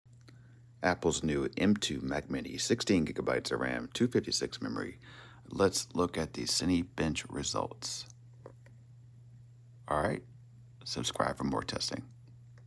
Apple M2 Mac Mini 16gb RAM 256gb Cinebench r23
Testing the Mac Mini M2 with 16 GB of ram and 256 GB memory.